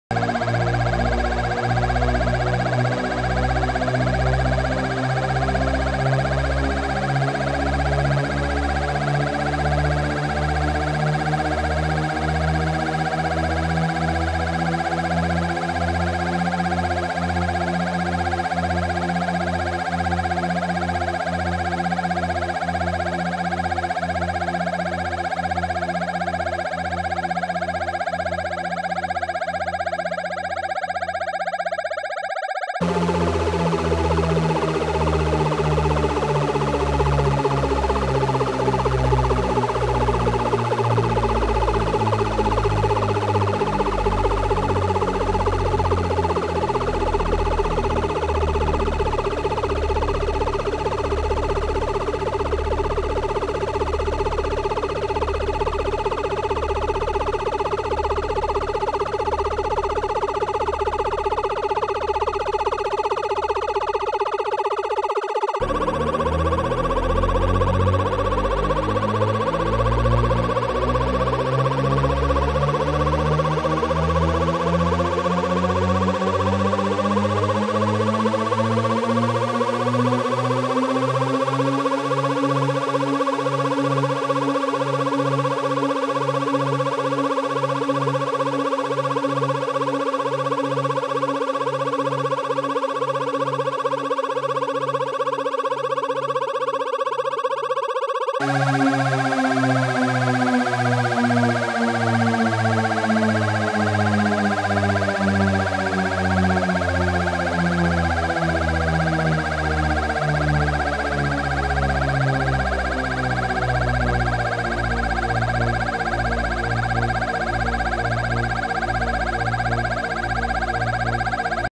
Two Emergencynths Synced Together
A slow orbit. The synth on the left is driving the on the one on the right which has its lfo all the way up.